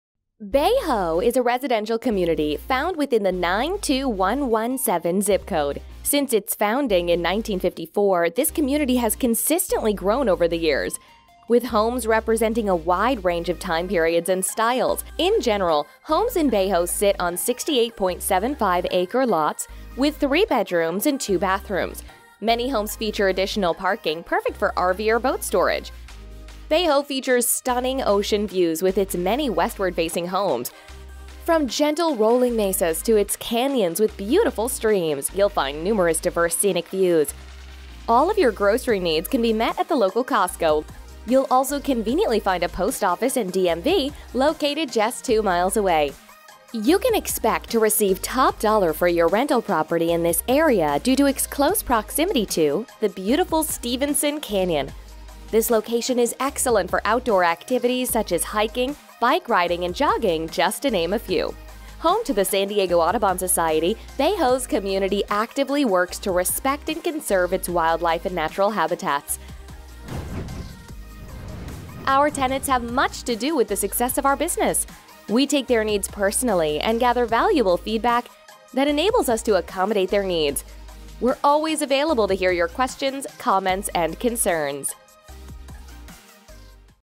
Bay Ho San Diego
Bay Ho is a residential community found within the 92117 Zip Code. Since its founding in 1954, this community has consistently grown over the years. This is a varied community, with homes representing a wide range of time periods and styles. In general, homes in Bay Ho homes sit on 68.75-acre lots with three bedrooms and two bathrooms. Many homes feature additional parking perfect for RV or boat storage. bay ho features stunning ocean views with its many westward-facing homes. From gentle rolling mesas to its canyons with beautiful streams, you will find numerous diverse scenic views. This area offers many amenities as well as delicious restaurants such as ISA.BELLA Pizzeria, Blue Door Urban Winery, Sushi Fish Attack, Mister Falafel and many more! You will also conveniently find a post office and DMV location just 2 miles away. One often visited local attraction is the beautiful Stevenson Canyon. This public park offers excellent outdoor activities such as hikers, bikers, skateboards, and joggers just to mention a few. Home to the San Diego Audubon Society, Bay Ho’s property owners, and tenants Respect and Conserve its wildlife and natural habitats.